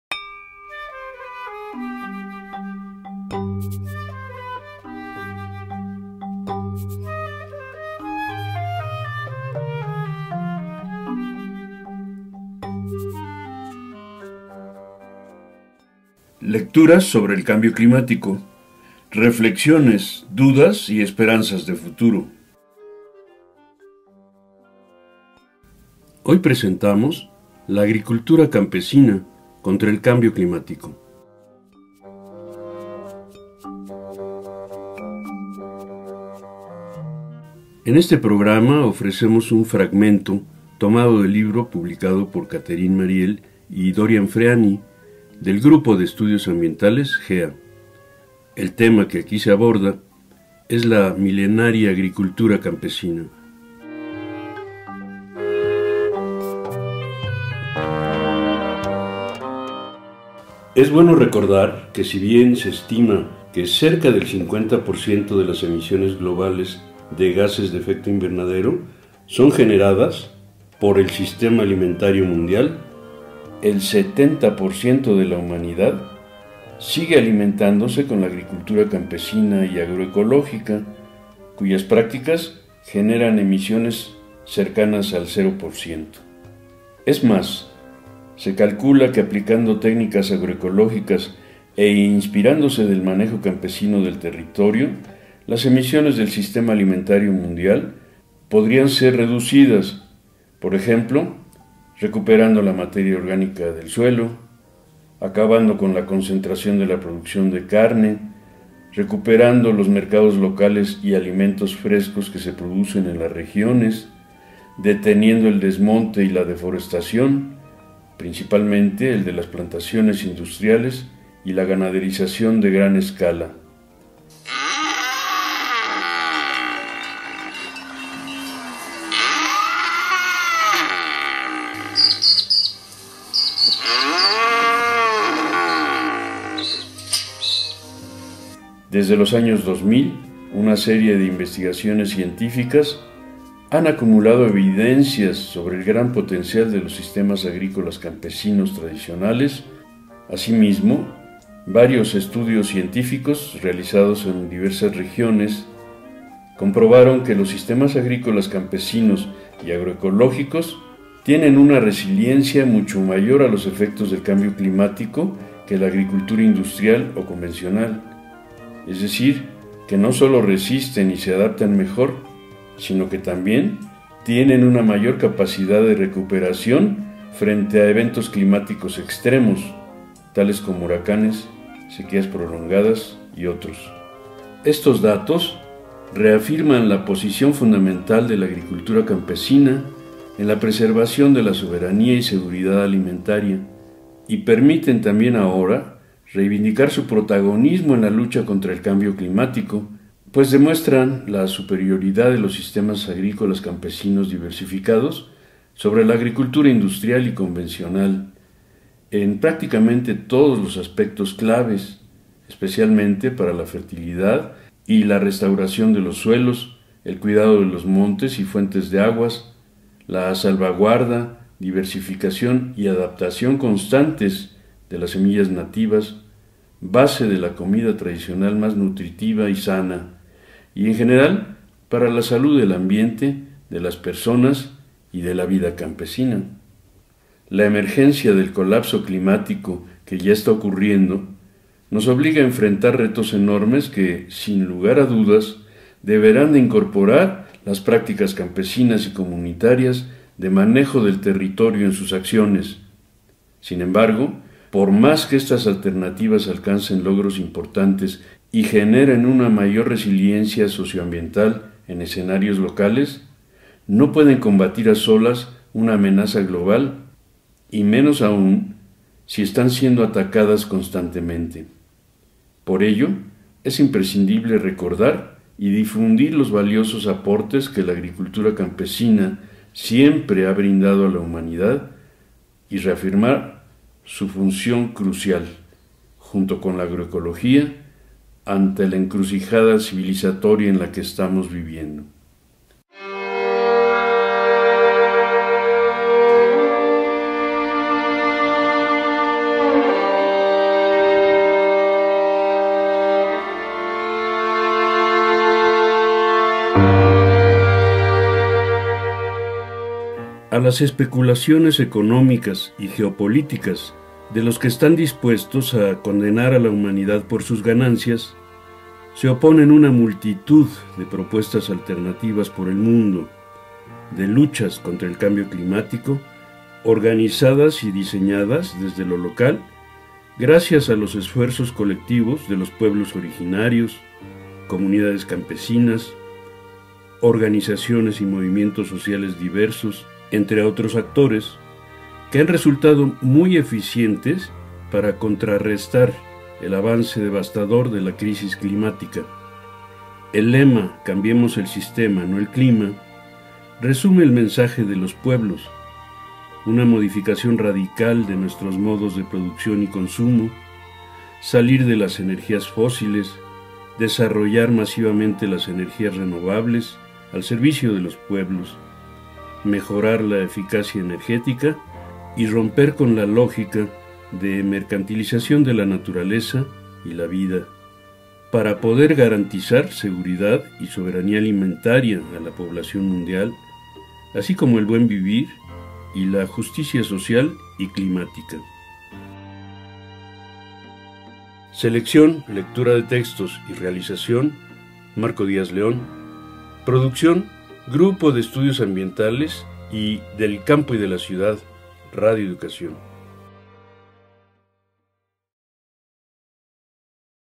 6. La agricultura campesina contra al cambio climático / Lecturas sobre el cambio climático por GEA
1. Lecturas sobre el cambio climático por GEA

6. La agricultura campesina contra al cambio climático
(Políticas globales. GEA 2019)

1. Lecturas sobre el cambio climático

2. Testimonios campesinos sobre los cambios climáticos

Créditos:

Producción
GEAVIDEO

Catherine Marielle

Mexico-Tenochtitlan 2019.